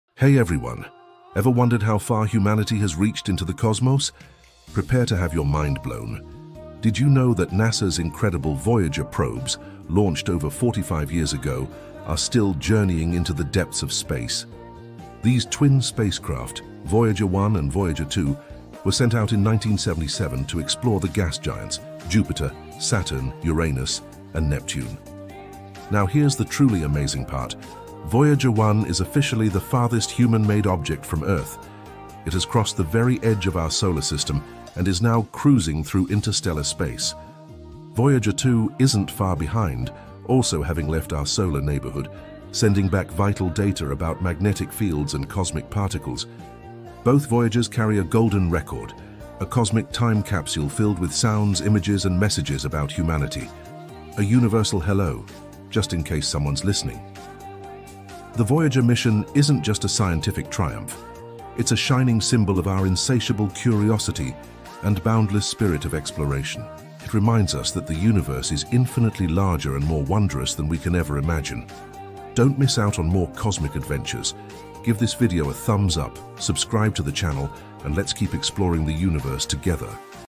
How Far Has Voyager Gone? NASA’s Interstellar Journey Explained#space universe #cosmos
How far has NASA’s Voyager spacecraft traveled? Launched in 1977, Voyager 1 and 2 have journeyed billions of miles beyond our solar system, exploring planets, moons, and now entering interstellar space. Discover the incredible distance these spacecraft have traveled, the amazing discoveries they’ve made along the way, and how they continue to send valuable data back to Earth after more than four decades.🌌space